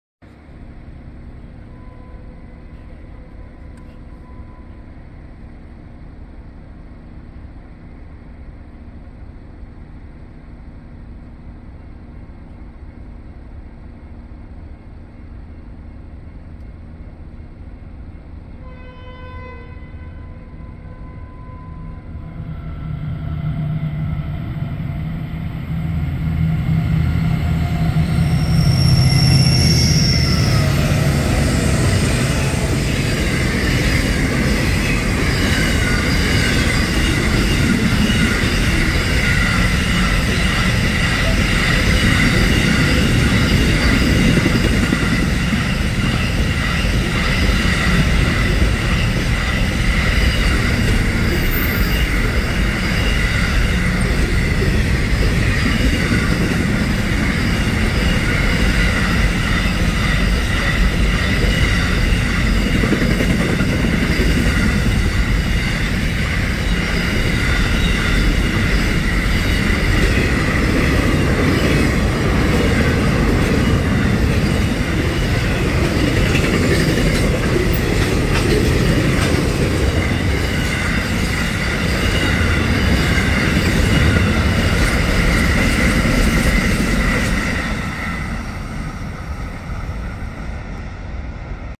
3AM9 SCT intermodal & steel to Melbourne, 18.08, 18/6/24, Stawell VIC
3AM9 Penfield SCT Freight Terminal, Adelaide SA to Laverton SCT Freight Terminal, Melbourne VIC.

SCT Logistics intermodal and steel freight train service led by CSR023, CSR006 & SCT006, hauling 87 wagon units (boxcars, refrigeration units, containers, more boxcars, flatpack steel slab wagons, steel cradles & Bluescope steel containers).

Stawell VIC, 6.08pm Tuesday 18th June 2024.

(Note no butterbox steel coil containers).